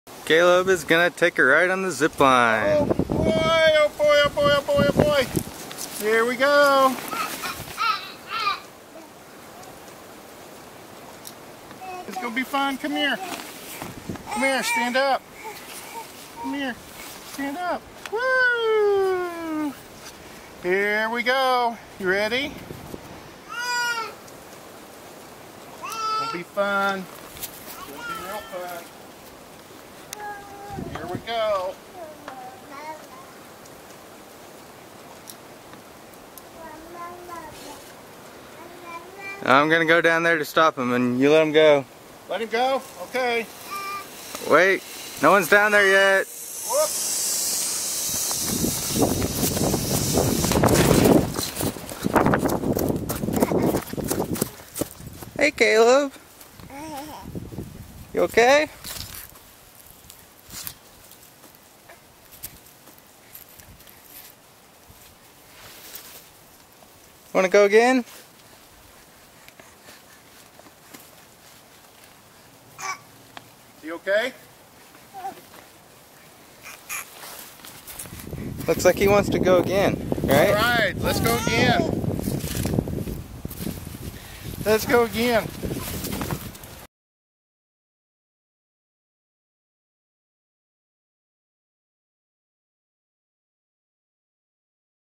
Caleb's first zipline ride and crash
Uncle Bryce and Pops take Caleb for a zip-line ride.